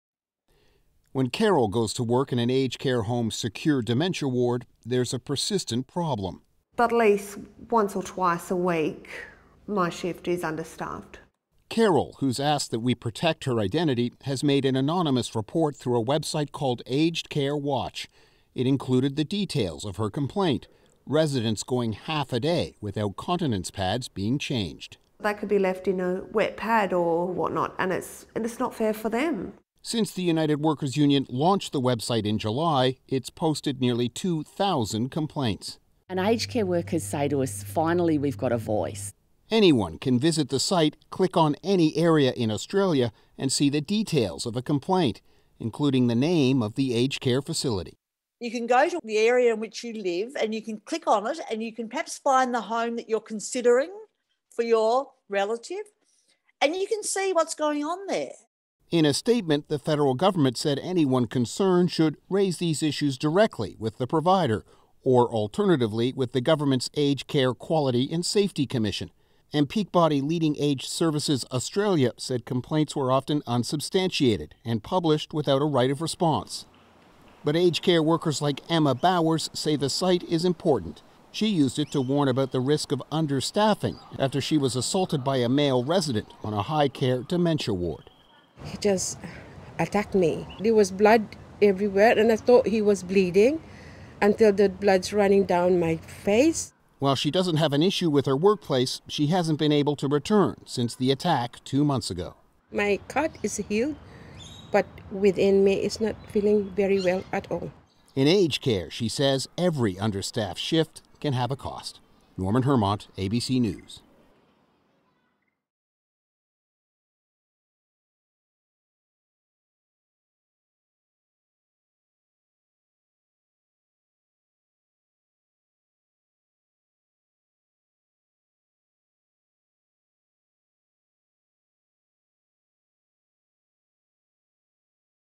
New aged care complaints website welcomed by advocates | ABC News
Aged care providers worry the complaints may be unsubstantiated and are published without a right of reply.